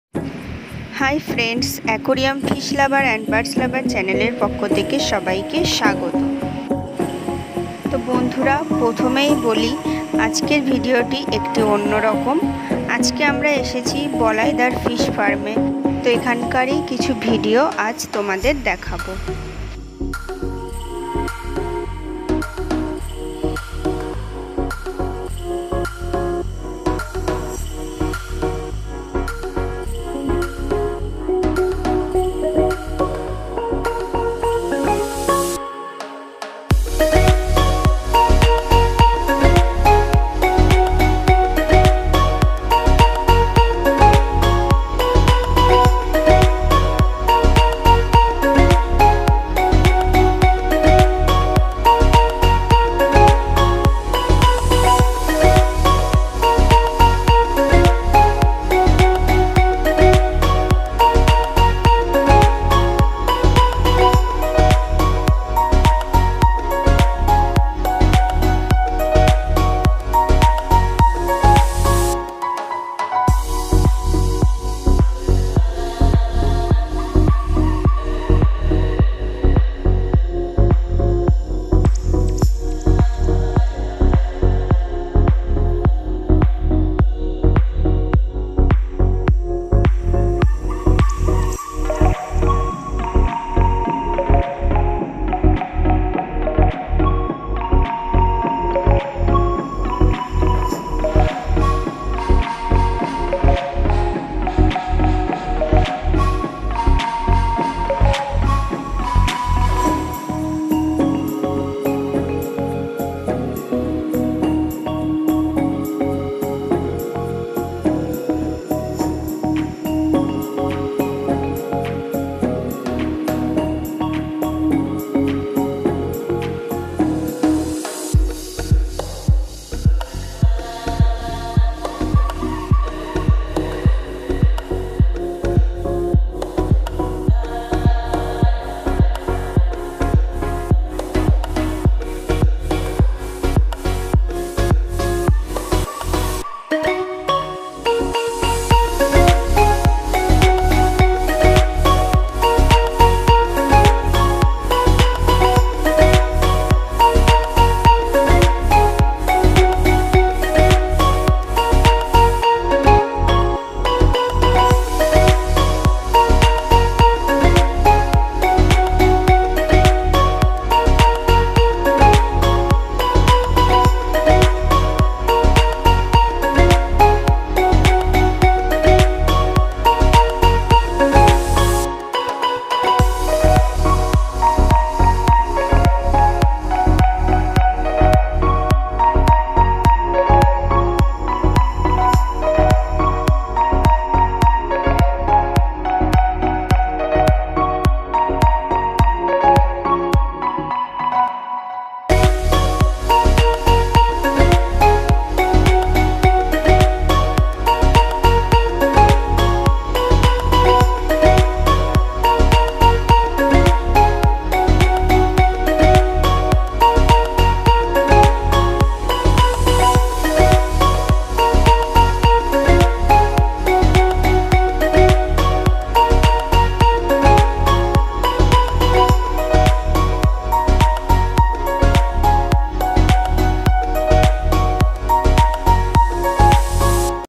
Fish farm visit / Goldfish, koi carp, ryukin, zebra fish, milky, red cap
Balai dar fish farm /
ryukin, goldfish, zebra fish, koi carp, milky, red cap,

আমরা বলাই দার মাছের ফার্ম এ গিয়েছিলাম
সেখানকার কিছু মাছের ভিডিও তোমাদের সাথে শেয়ার  করলাম।